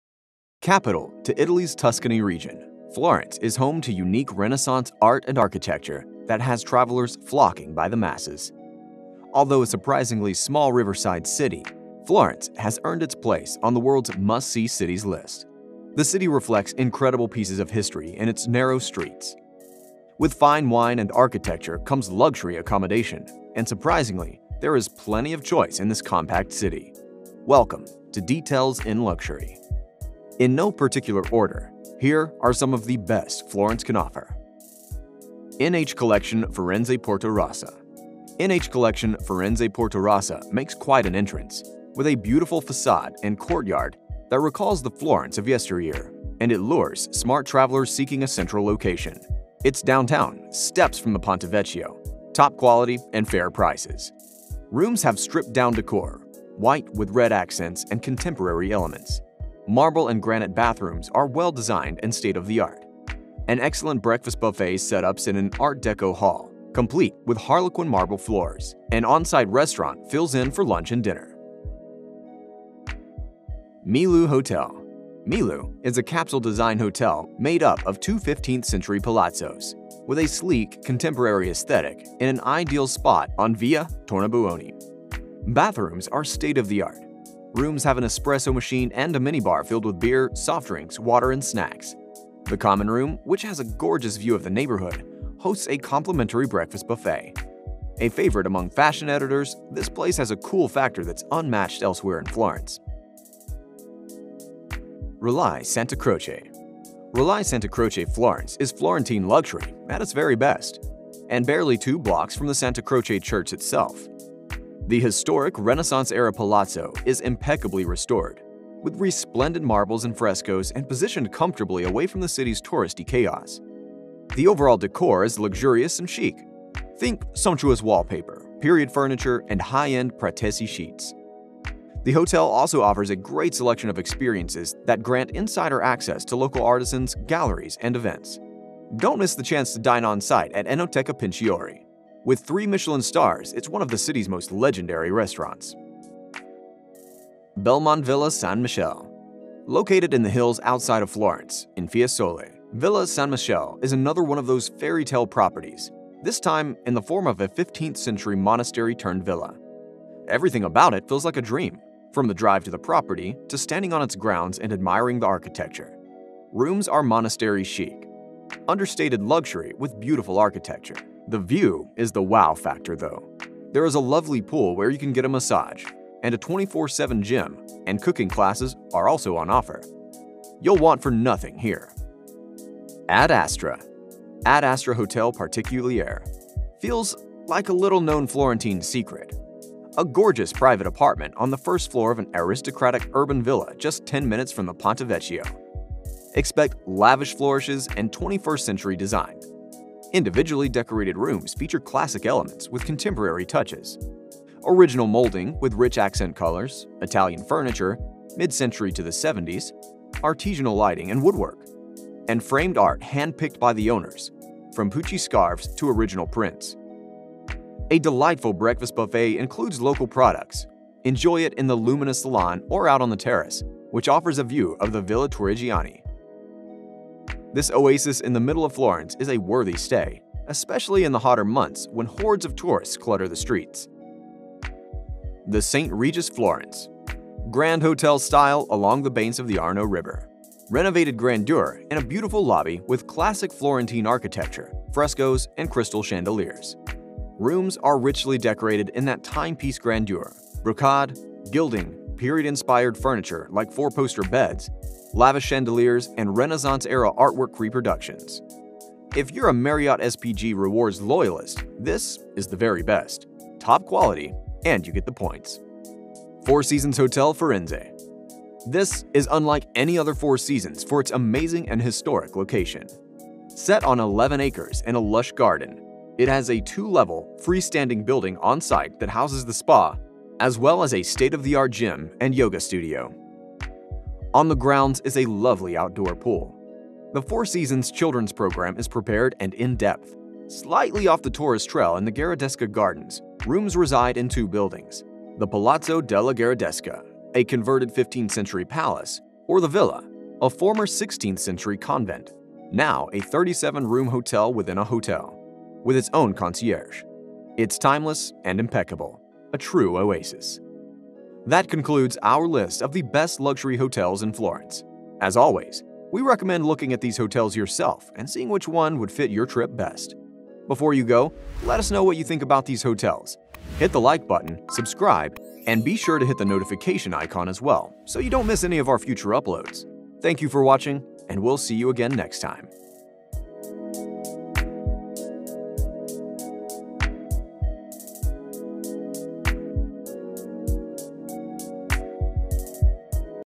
Top 7 Best Hotels In Florence | Luxury Hotels In Florence, Italy
In this video we are going to show the list of the best and most luxurious hotels in Florence Italy, Florence is a beautiful and well known city around the world, so we are going to show the best hotels to stay in Florence.

Hope this video helps you to decide where to stay if you plan on visiting Florence Italy.